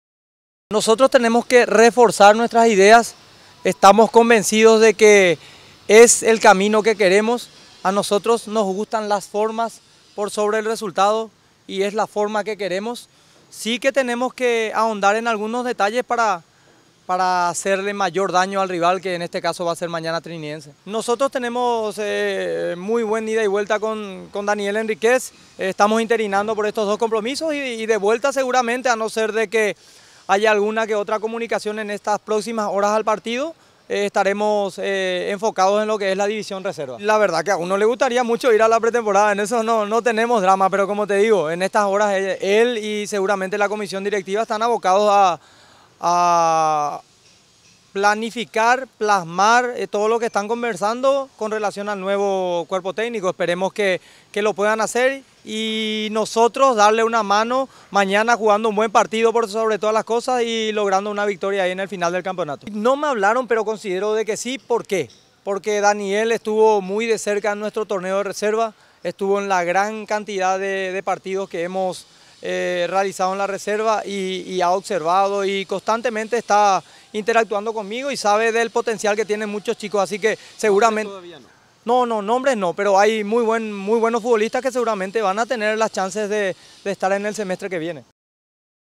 Previa Cerro Porteño vs. Sportivo Trinidense - Torneo Apertura 2017 - Jorge Núñez
Jorge Martín Núñez completa este jueves su interinato en Cerro Porteño, enfrentando a Sportivo Trinidense en partido de la fecha 22 del torneo Apertura. Paralelamente, Cerro Porteño busca entrenador para el segundo semestre del año.